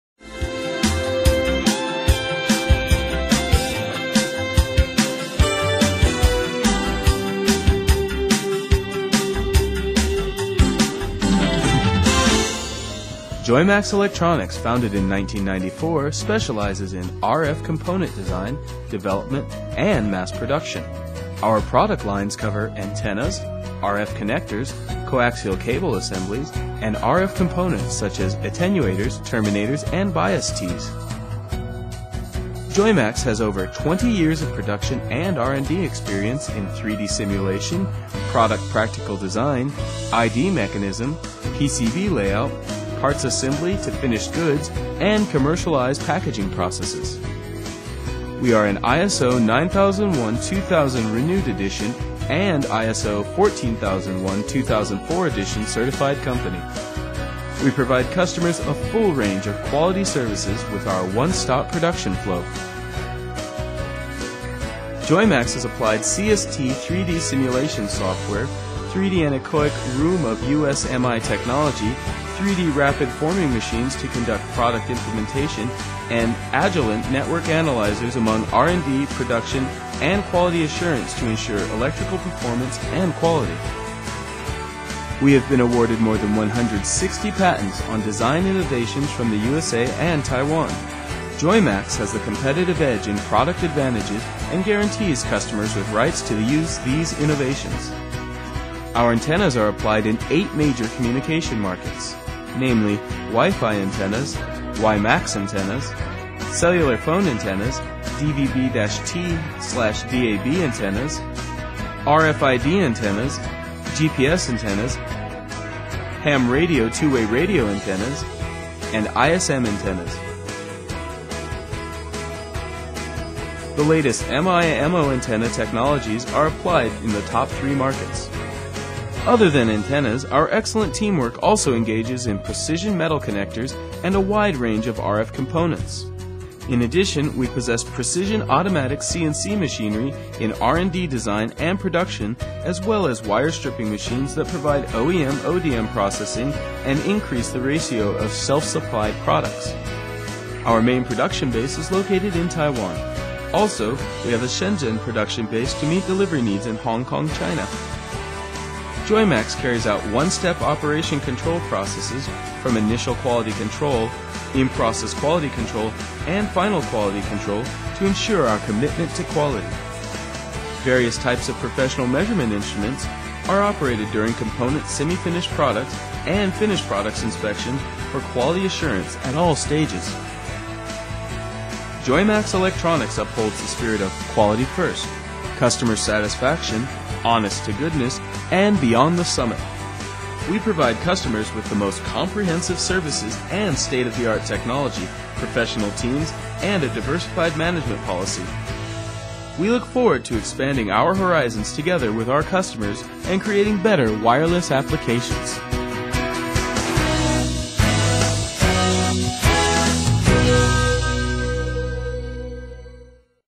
士誼科技事業股份有限公司Joymax Electronics Co., Ltd.,
Joymax Electronics Co., Ltd. was founded in 1995, has been keeping the leading edge in the industry by continuously investing in R&D resource of art facilities for antenna test and measurement. We offer full design and development capabilities because our expertise and equipment infrastructure enable to produce fast and accurate prototypes for your project. The spirit of Joymax is to establish customer satisfaction, product reliability and continuous product innovation for future market evolution.
Joymax designs and manufactures antenna and accessory products for the 9 key markets that covers the wireless spectrum for today's major wireless communication applications:

* Wireless LANs
* WiMAX
* Cellular Phone
* Ham Radio
* GPS
* RFID
* DAB / DMB
* DVB-T
* ISM Band

In addition to cable assemblies, we also provide connectors and mounting brackets designed to make the installation of our antennas more cost-effective.

To meet more and more rapid request for new design from our customers, Joymax consigned MI Technologies, worlds leading supplier of all instrument used to accomplish Microwave Antenna Measurement, to build 3D chamber. The chamber helps shorten research and development time, enhance the product reliability. Most importantly, it brings more customer credibility.